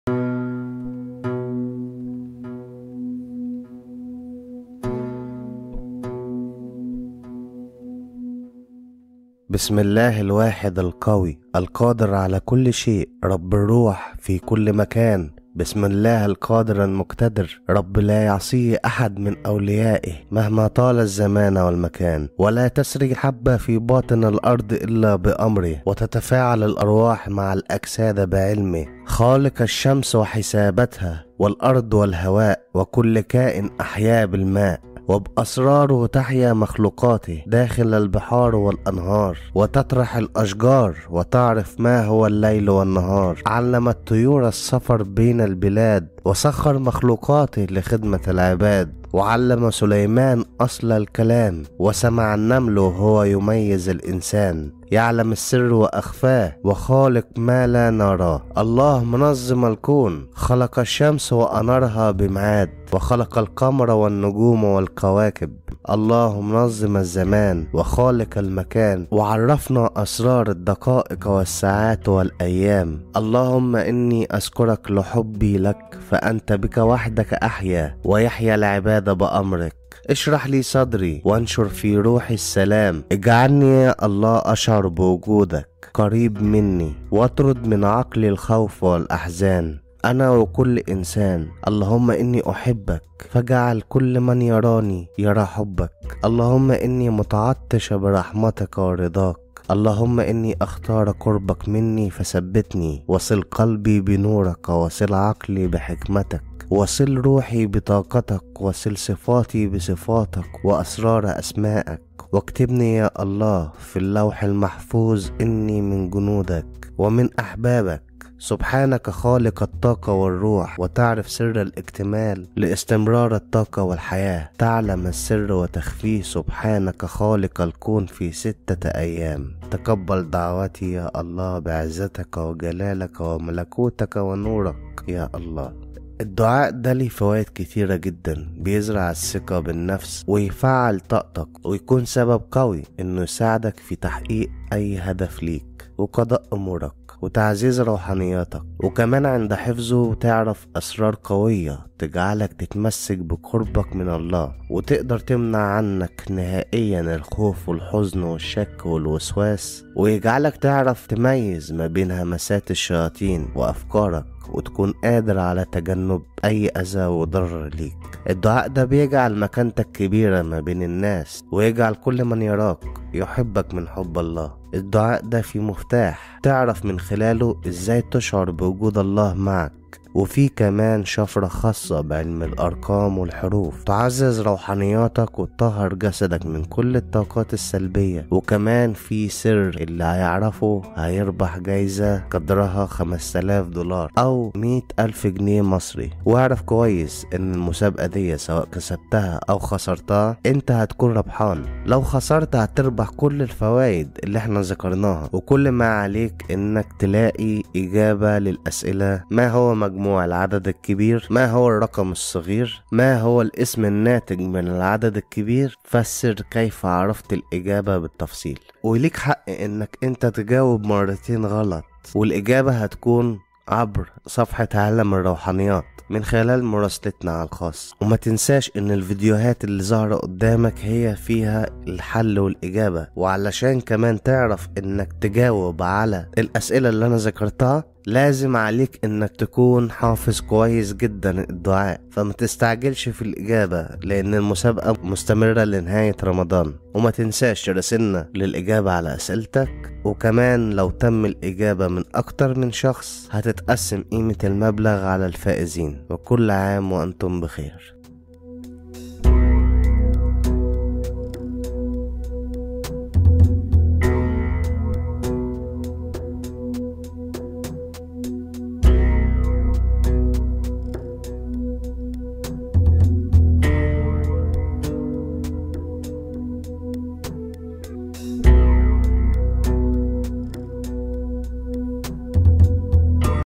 علم الحروف والارقام الذات العليا
بسم الله الواحد القوى القادر على كل شي رب الروح في كل مكان بسم الله القادر المقتدر رب لا يعصيه احد من اوليائه مهما طال الزمان والمكان ولا تسري حبة في باطن الأرض الا بامره وتتفاعل الأرواح مع الأجساد بعلمه  خالق الشمس وحسابتها والأرض والهواء وكل كائن احياه بالماء وباسراره تحيا مخلوقاته داخل البحار والانهار وتطرح الأشجار وتعرف ما هو الليل والنهار علم الطيور السفر بين البلاد وسخر مخلوقاته لخدمة العباد وعلم سليمان اصل الكلام وسمع النمل وهو يميز الانسان  يعلم السر واخفاه وخالق ما لا نراه الله منظم الكون خلق الشمس وانارها بميعاد وخلق القمر والنجوم والكواكب الله منظم الزمان وخالق المكان وعرفنا اسرار الدقائق والساعات والأيام اللهم اني اذكرك لحبي لك فانت بك وحدك احيا ويحيا العباد بامرك اشرح لي صدرى وانشر في روحى السلام اجعلنى يا الله اشعر بوجودك قريب منى واطرد من عقلى الخوف والاحزان انا وكل اانسان اللهم انى احبك فاجعل كل من يرانى يرى حبك اللهم انى متعطش برحمتك ورضاك اللهم انى اختار قربك منى فـثـبـتـنى وصل قلبي بنورك وصل عقلي بحكمتك وصل روحى بطاقتك وصل صفاتى بصفاتك واسرار اسمائك واكتبنى يا الله في اللوح المحفوظ انى من جنودك ومن احبابك سبحانك خالق الطاقة والروح وتعرف سر الاكتمال لاستمرار الطاقة والحياة تعلم السر وتخفيه سبحانك خالق الكون في ستة أيام تقبل دعوتى يا الله بعزتك وجلالك وملكوتك ونورك يا الله
هذا دعاء يزرع الثقة بالنفس ويفعل طاقتك ويكون سبب في مساعدتك في تحقيق اهدافك وقضاء امورك وتعزيز روحانياتك وأيضا عند حفظه تعرف اسرار قوية تجعلك تتمسك بقربك من الله وتمنع عنك نهائيا الخوف والحزن والشك والوسواس ويجعلك تعرف همسات الشيطان وتكون قادر على تجنبها هذا الدعاء يجعل مكانتك كبيرة جدا وكل من يراك يحبك هذا الدعاء فيه مفتاح تعرف كيف تشعر بوجود الله معك وفيه أيضا شفرة خاصة بعلم الأرقام والحروف تعزز روحانياتك وتطهر جسدك من كل الطاقات السلبية وأيضا فيه سر من يعرفه سوف يربح جائزة بمبلغ 5000 دولار او 100000 جنيه مصرى واعلم جيدا ان هذه المسابقة اذا خسرتها سوف تكون ربحت كل ما شرحته عن فوائد الذكر
كل ما عليك ان تجد الإجابة لهذه الاساله
ما هو مجموع العدد الكبير   (مجموع الأرقام المطلوبة جمعها )
ما هو الرقم الصغير          (مجموع بسط الرقم الكبير )
ما هو الاسم الناتج من العدد الكبير  (اسم من أسماء الله الحسنى )
فسر كيف عرفت التفاصيل
سوف أساعدك ببعض الفيديوهات التي تدلك على الإجابة
حتى تعرف الإجابة يلزم عليك حفظ الدعاء المذكور في الأعلى فلا تتعجل بالاجابة فان المسابقة مستمرة لنهاية شهر رمضان
الإجابة عبر مراسلتنا على الخاص ولك حق الإجابة الخطاء مرتين فقط واذا تم الإجابة على اكتر من شخص سوف تنقسم قيمة المبلغ على الفائزين وكل عام وانتم بخير
التواصل عبر القيس بوك